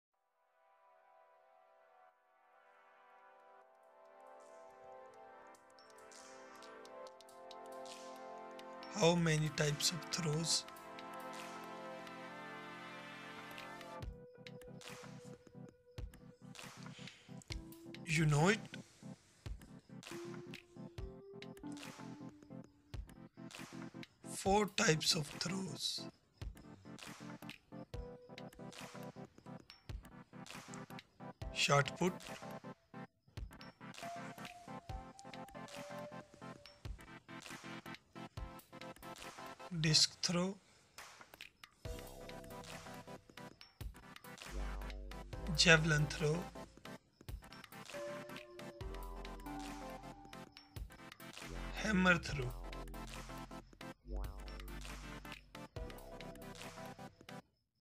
How many types of throws
There are four types of through in Olympics game . Discus throw shot put javelin and hammer throw all through part of the Olympics game.